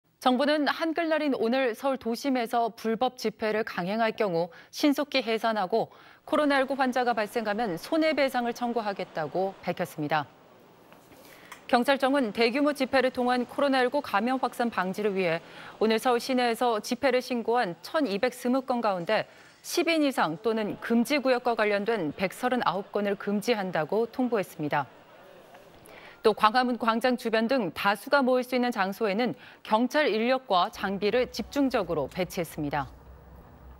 한글날 불법집회 금지…코로나 전파 시 손해배상 청구 / SBS
정부는 한글날인 오늘(9일) 서울 도심에서 불법 집회를 강행할 경우 신속히 해산하고, 코로나19 환자가 발생하면 손해배상을 청구하겠다고 밝혔습니다.
경찰청은 대규모 집회를 통한 코로나19 감염 확산 방지를 위해 오늘 서울 시내에서 집회 신고한 1천220건 가운데 10인 이상 또는 금지구역과 관련된 139건을 금지한다고 통고했습니다.
또 광화문광장 주변 등 다수가 모일 수 있는 장소에는 경찰 인력과 장비를 집중적으로 배치했습니다.